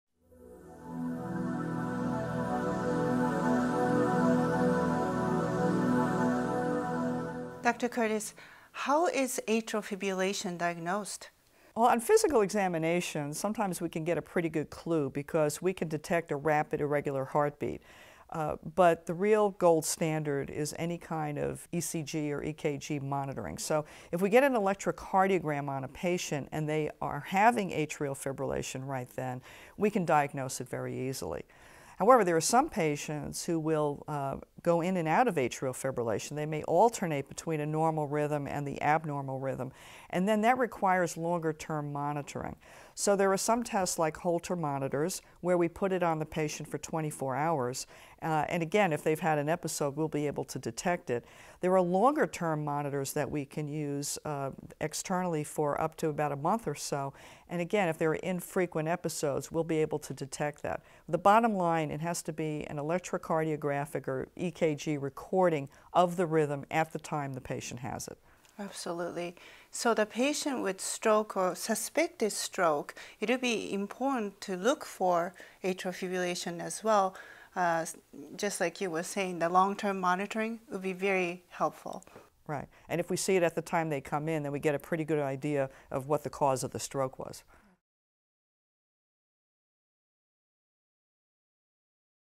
How is atrial fibrillation diagnosed?
Expert cardiologists Dr. Anne Curtis and Dr. Hiroko Beck explain the ways that atrial fibrillation is diagnosed, including physical examination, assessment of signs and symptoms, and results from an electrocardiogram test (also called ECG or EKG) that detects atrial fibrillation at the time it is happening. For patients who have intermittent or infrequent atrial fibrillation, a 24 hour Holter monitor or a longer term event recorder may be used to monitor heart rhythm and detect atrial fibrillation.